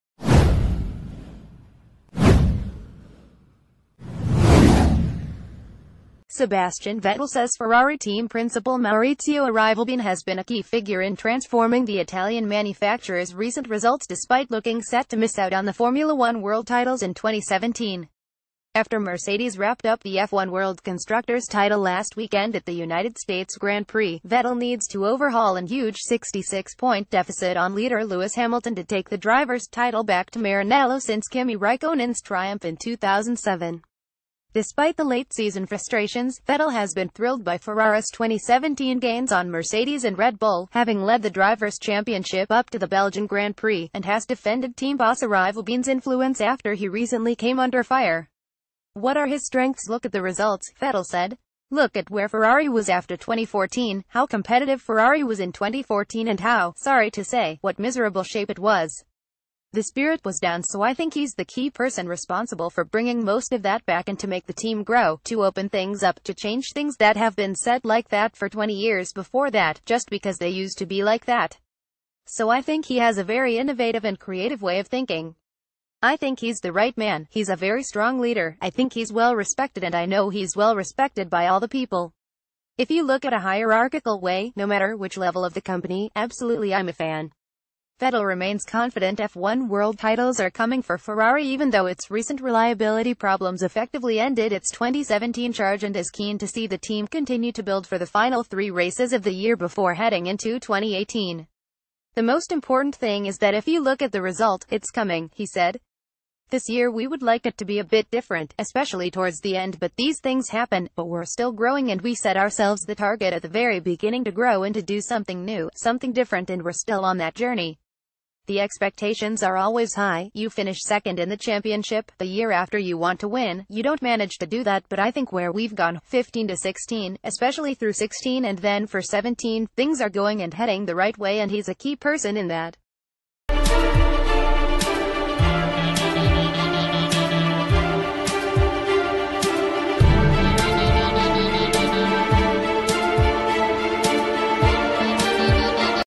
F1 | Vettel: ferrari were in miserable shape before arrivabene
Vettel: ferrari were in miserable shape before arrivabene

Sebastian Vettel says Ferrari team principal Maurizio Arrivabene has been a key figure in transformi...